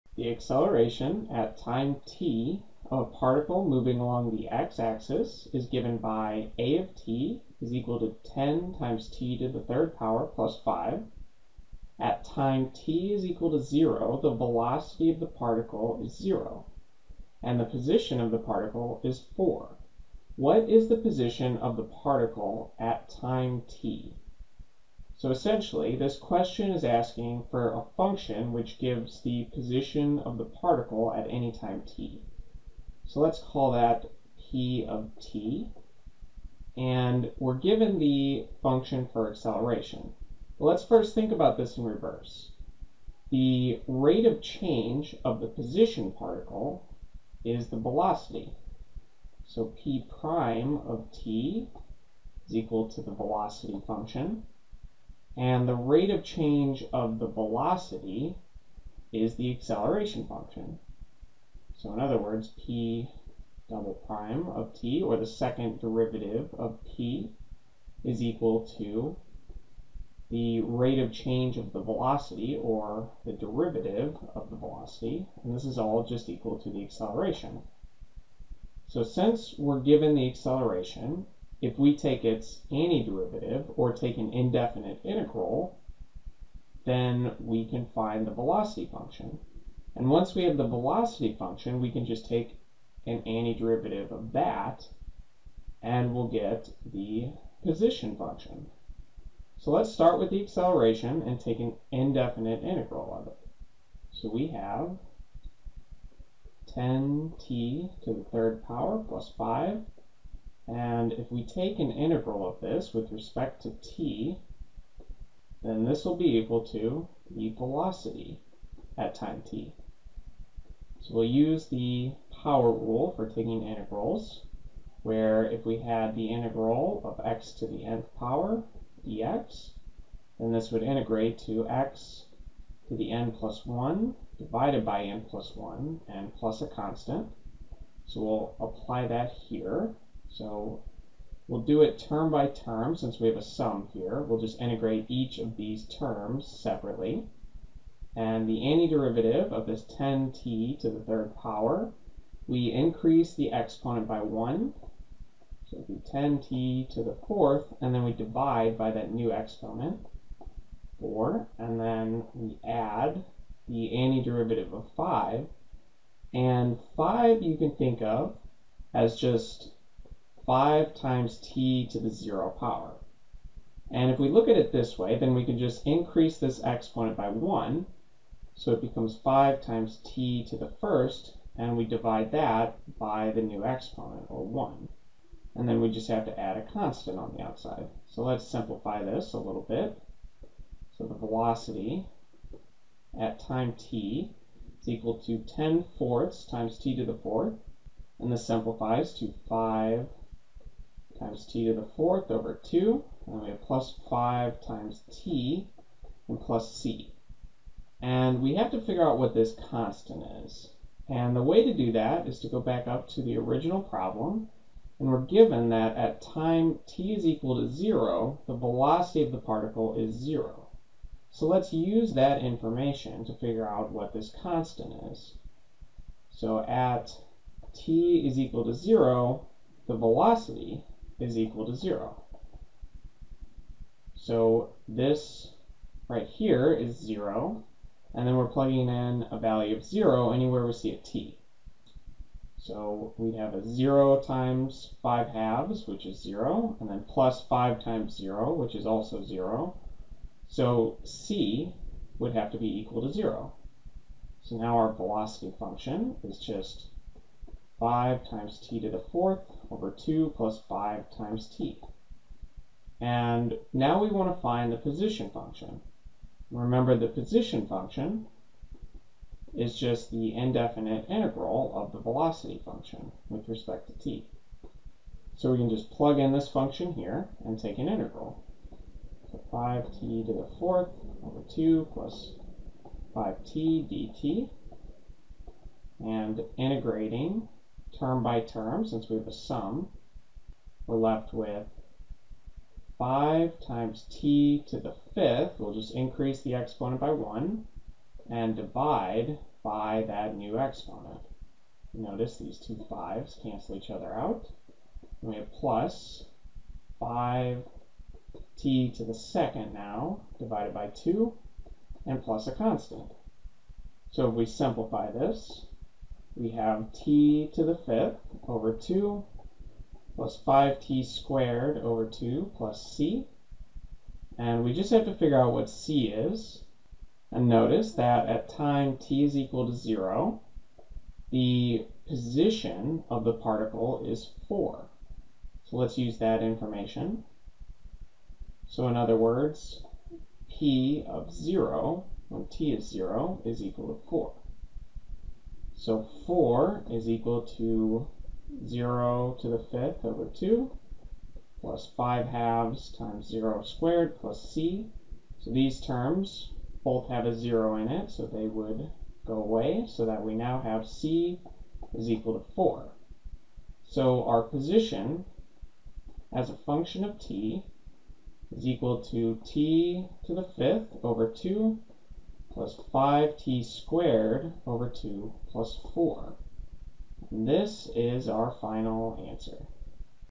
CLEP Calculus: Acceleration, Velocity, and Position
This is the 9th problem on the 1st CLEP calculus practice test.